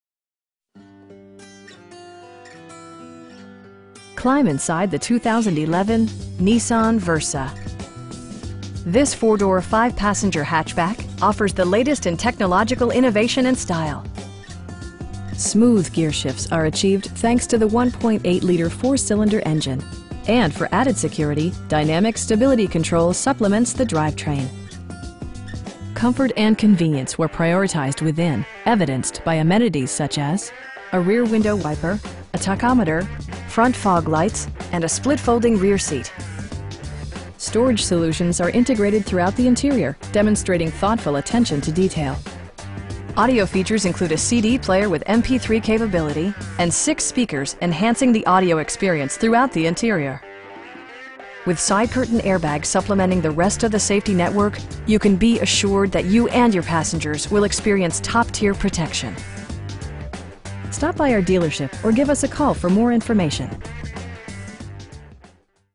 2011 Nissan Versa 1.8SL in Grapevine, TX 76051
Texas Nissan
1401 W State Hwy 114

Climb inside the 2011 Nissan Versa. This 4 door, 5 passenger hatchback offers the latest in technological innovation and style. Smooth gearshifts are achieved thanks to the 1.8 liter 4 cylinder engine, and for added security, dynamic Stability Control supplements the drivetrain. Comfort and convenience were prioritized within, evidenced by amenities such as: a rear window wiper, a tachometer, front fog lights, power door mirrors, power windows, cruise control, and a split folding rear seat. Storage solutions are integrated throughout the interior, demonstrating thoughtful attention to detail Audio features include a CD player with MP3 capability, and 6 speakers, enhancing the audio experience throughout the interior. With side curtain airbags supplementing the rest of the safety network, you can be assured that you and your passengers will experience top-tier protection. We pride ourselves on providing excellent customer service. Stop by our dealership or give us a call for more information.